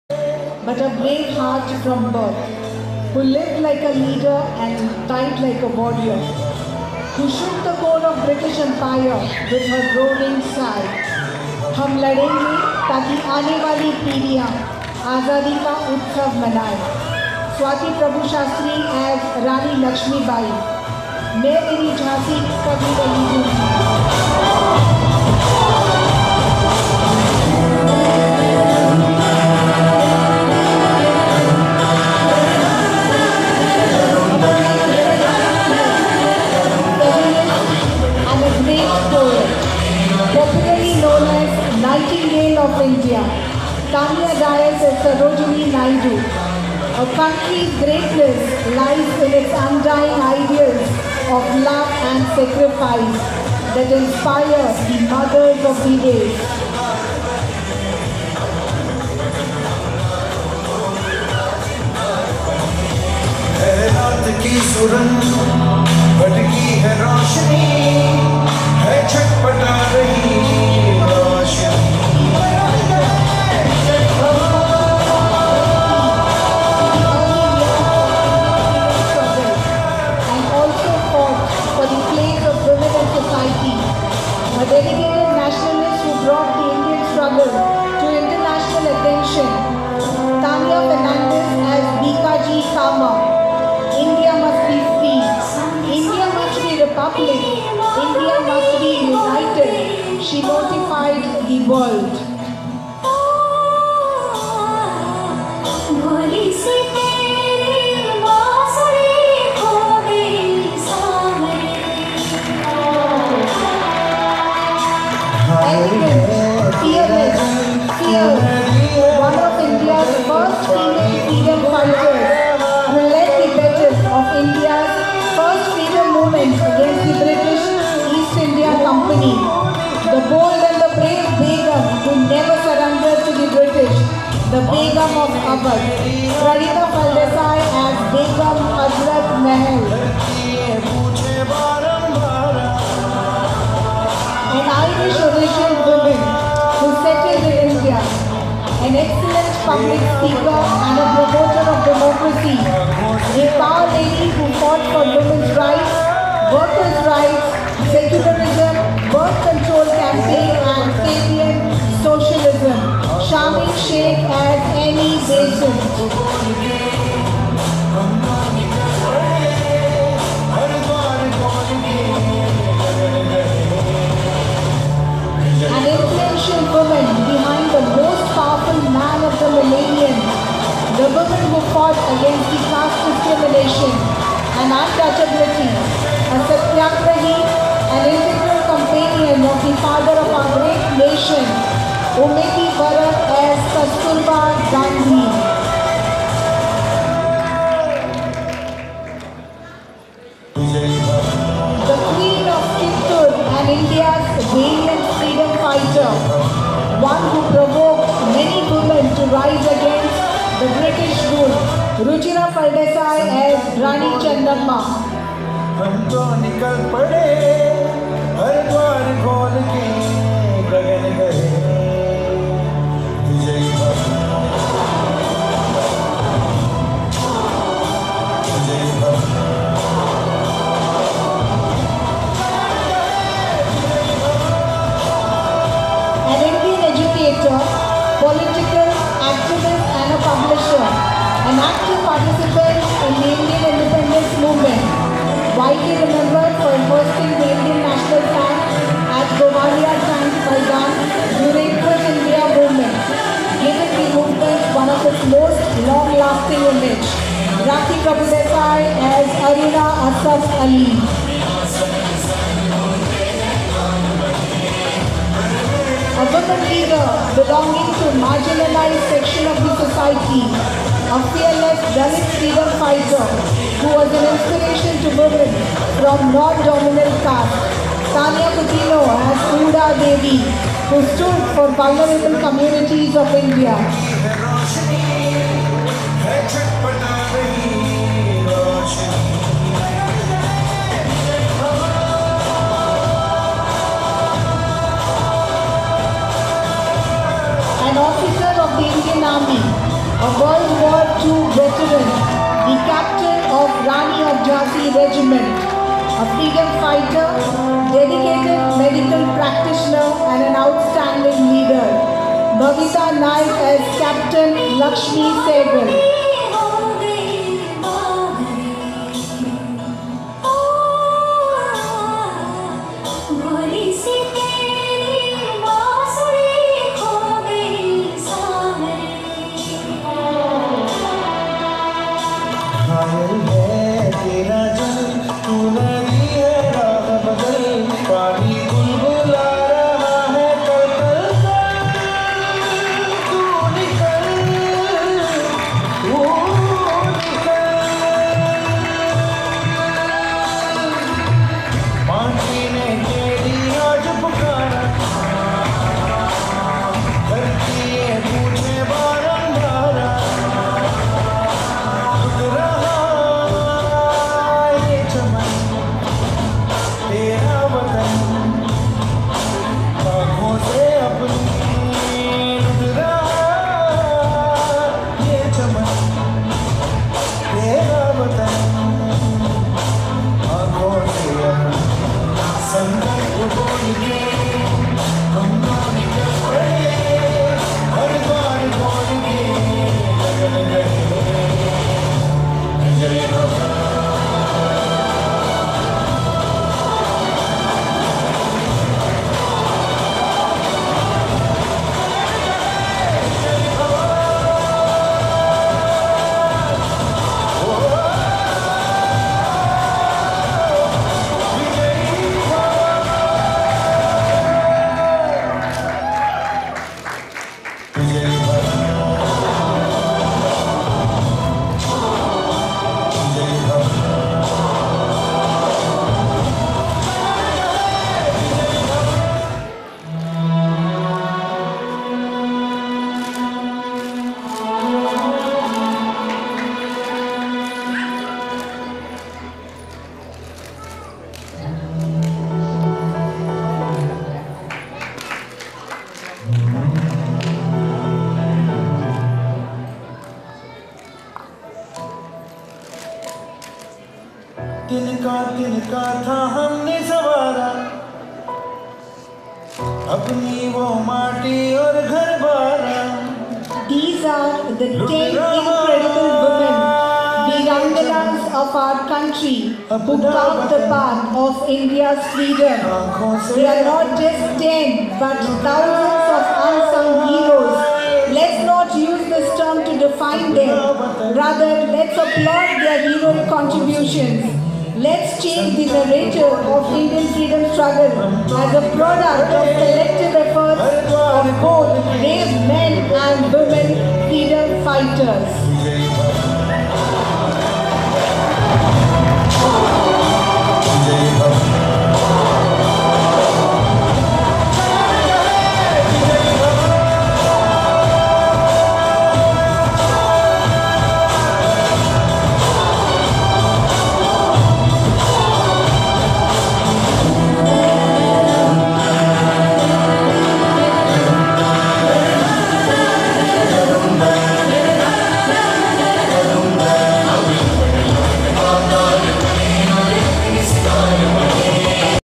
12/18 FASHION SHOW |GOVT. MULTIPURPOSE HIGHER SECONDARY SCHOOL, BORDA, MARGAO| KALA SRUJANOTSAV 2022
Performance by staff of GOVT. MULTIPURPOSE HIGHER SECONDARY SCHOOL, BORDA, MARGAO for Fashion Show Competition held at Ravindra Bhavan Margao under the banner of Kala Srujanotsav 2022 organised by Sarkari Karmachari Saunskrutik Manch Goa

Copyright credit to the original music audios used in the video